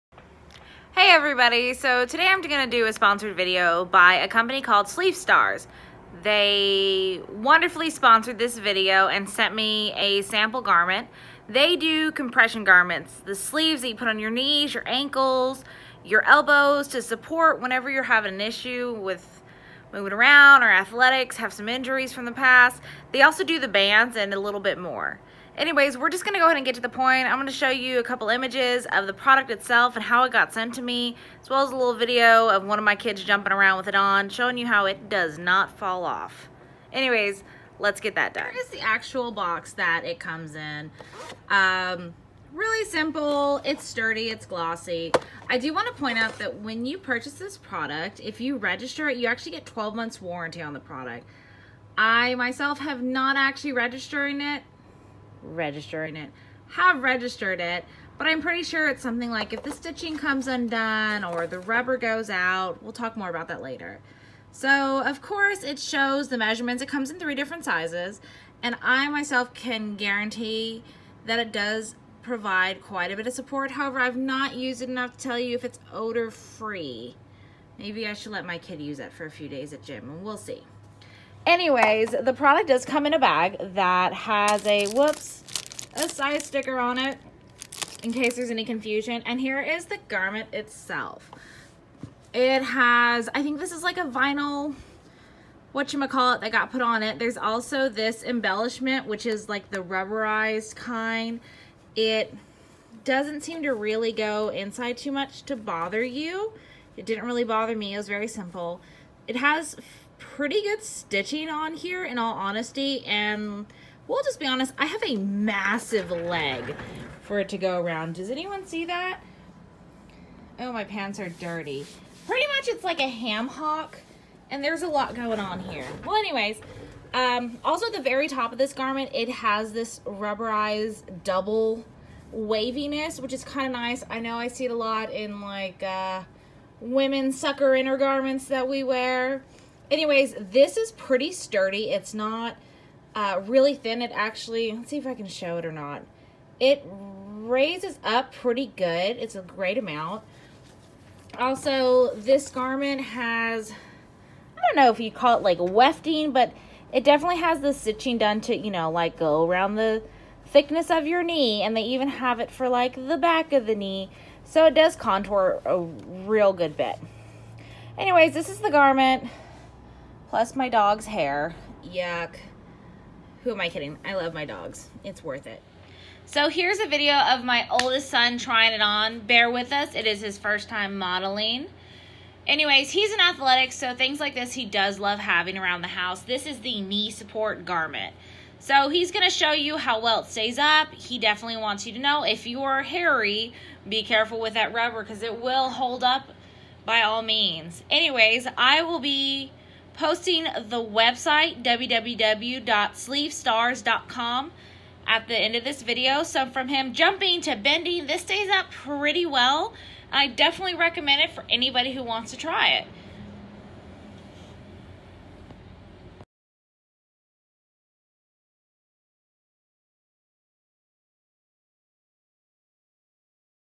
Sleeve Stars Knee Support Review
Sleeves stars knee support is a pretty impressive product. They gave me this project try out and I highly recommend it if the ones at the store just aren’t quite it out for you